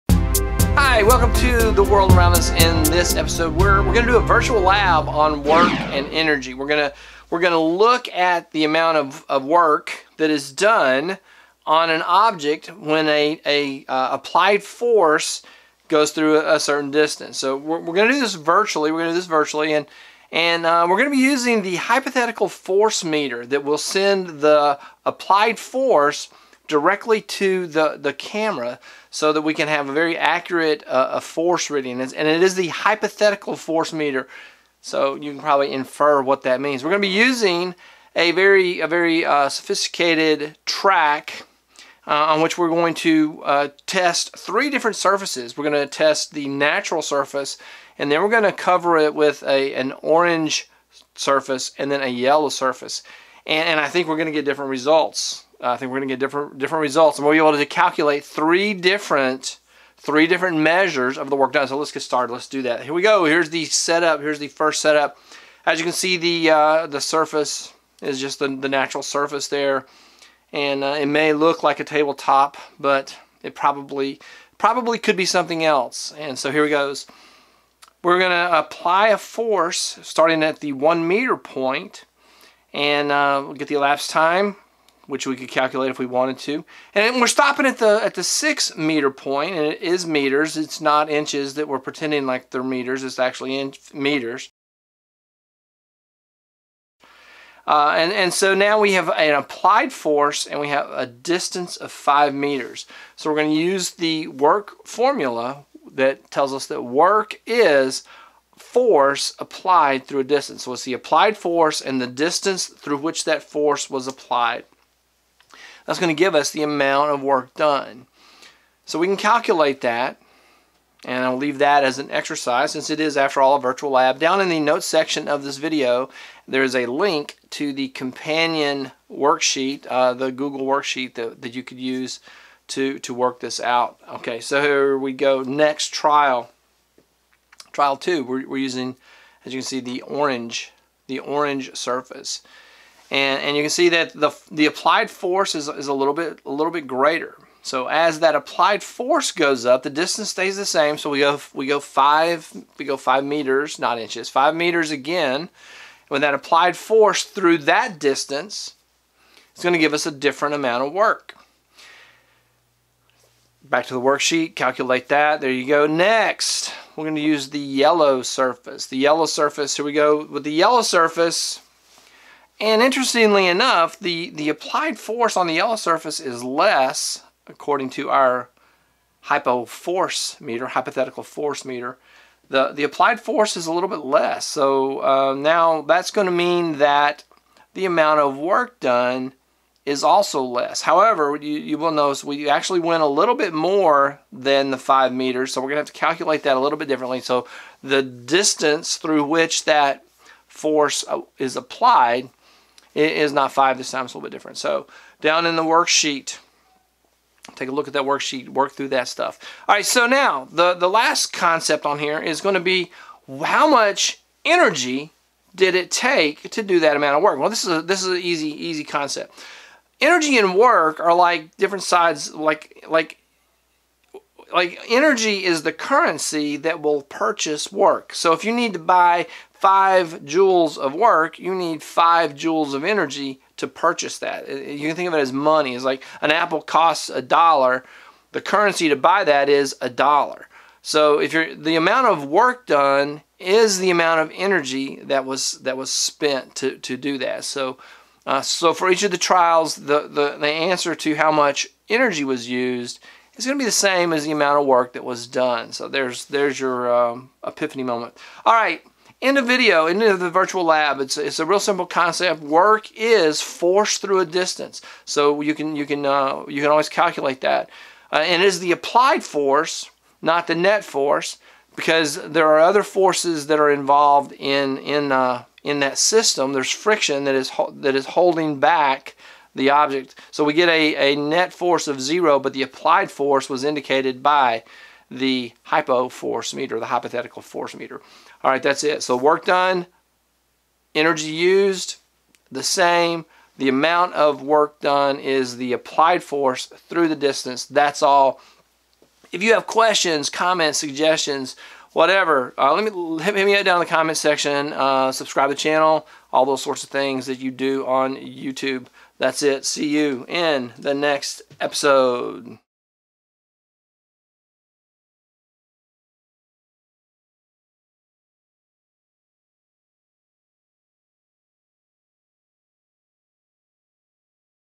Force, Distance, Work, and Energy: Virtual Lab
This video provides a virtual opportunity to work with applied forces through a distance in order to calculate work done.